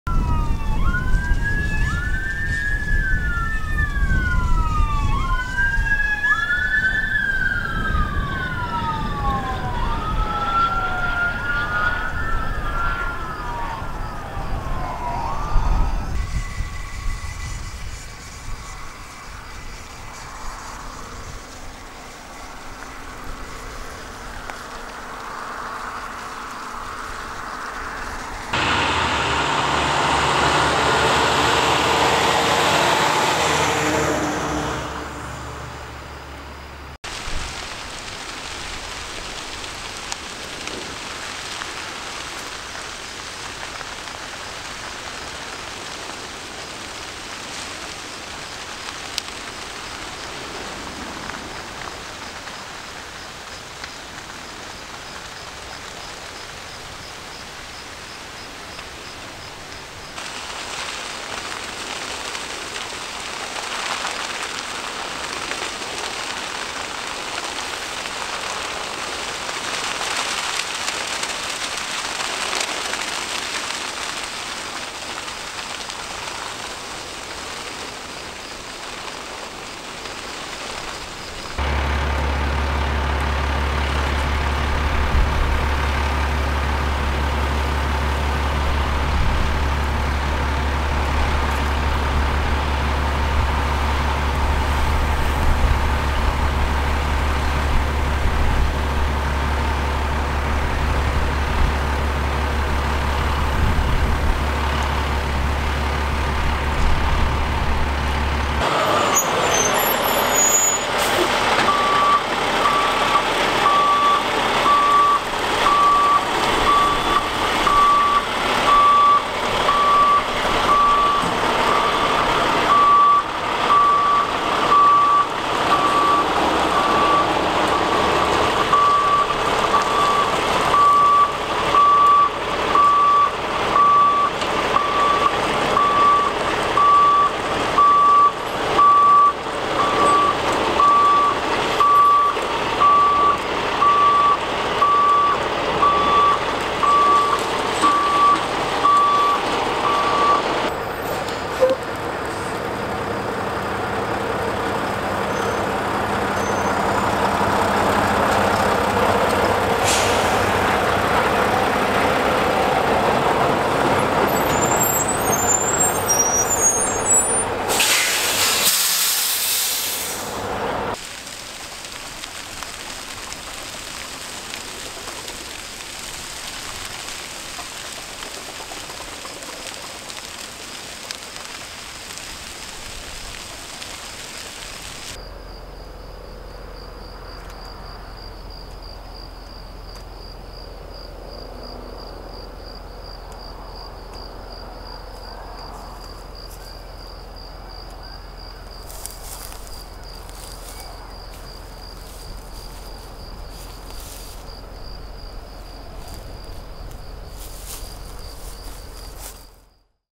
1,200 Acre Wildfire Destroys Structures, Evacuates Neighborhoods near Vacaville, CA.
The Quail Fire in rural Vacaville, California quickly grew to over 1,200 acres Saturday night, fueled by high winds and dry vegetation. The fire threatened over 100 structures, and destroyed three. Firefighting resources from across the state were assigned to the incident late Saturday night, June 6, 2020.